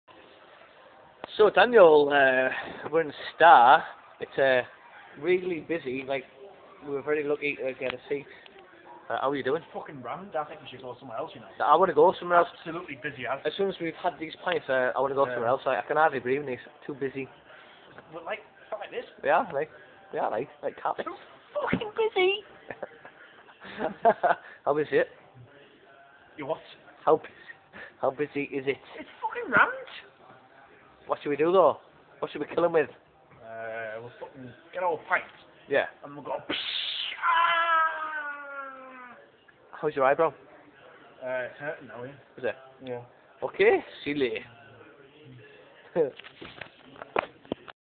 me and mortby in the star..
we went for a cheeky pint and thought we would show how quiet it was..